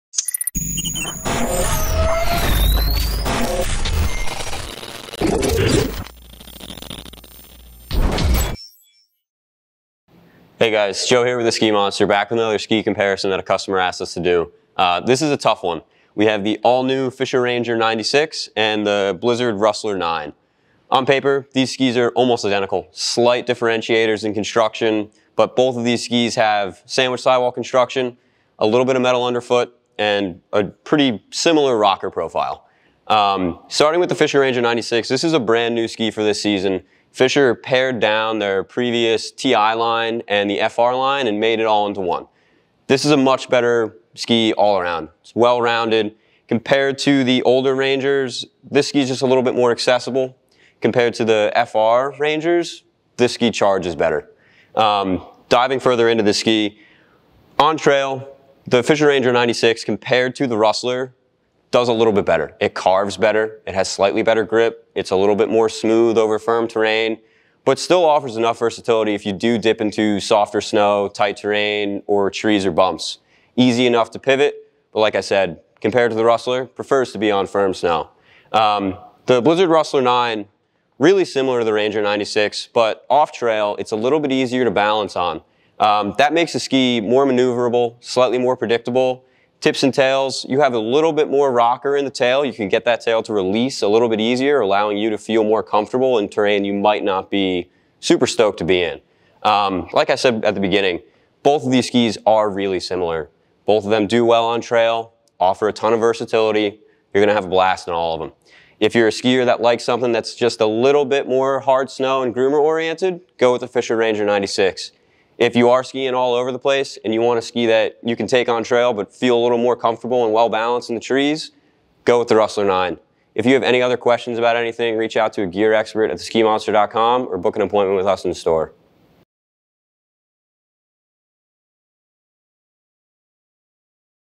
2023 Fischer Ranger 96 vs. Blizzard Rustler 9
Joe compares the new Fischer Ranger 96 and the Blizzard Rustler 9.